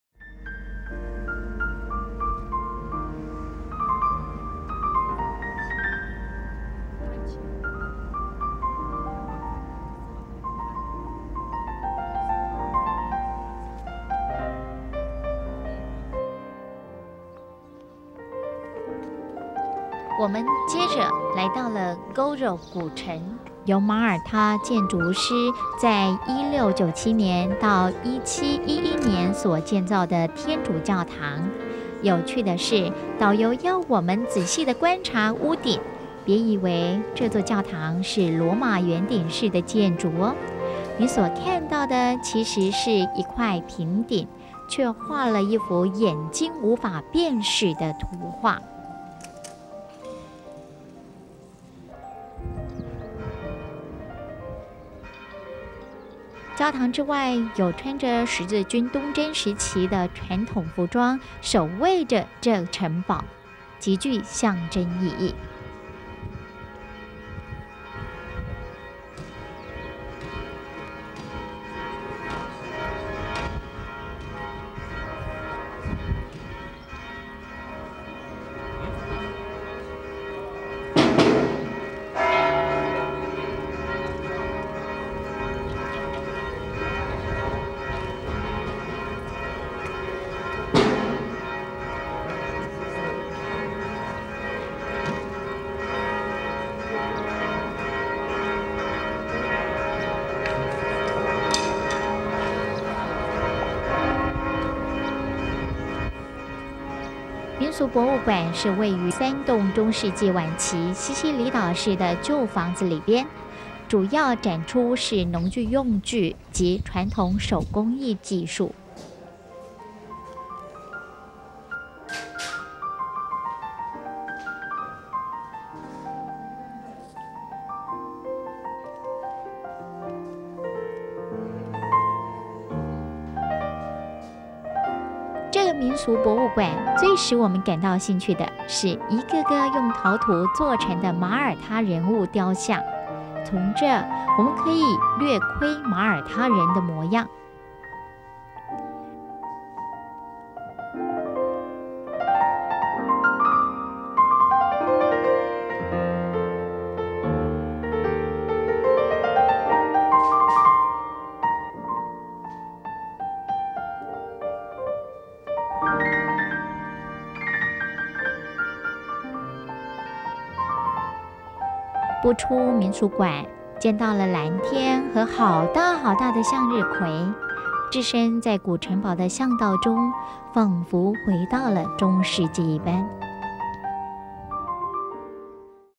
馬爾他(7) Gozo古城Citadella
我們接著來到Gozo古城Citadella，由馬爾他建築師在1697年到1711年所建造的天主教堂Cathedral of the Assumption，有趣的是，導遊要我們仔細的觀察屋頂，別以為這座教堂是羅馬圓頂式的建築喔，你所看到的其實是一塊平頂，卻畫了一幅眼睛無法辨識的圖畫。教堂之外有穿著十字軍東征時期的傳統服裝，守衛著這個城堡，極具象徵意義。民俗博物館Folklore Museum是三棟中世紀晚期西西里島式的舊房子裡邊，主要展出農具用具及傳統手工藝技術，這民俗博物館最使我們感到興趣的是，一個個用陶土做的人物雕像，從這兒我們可以略窺馬爾他人的模樣。步出民俗館，見到了藍天和好大好大的向日葵，置身在古城堡的巷道中，彷彿回到了中世紀一般。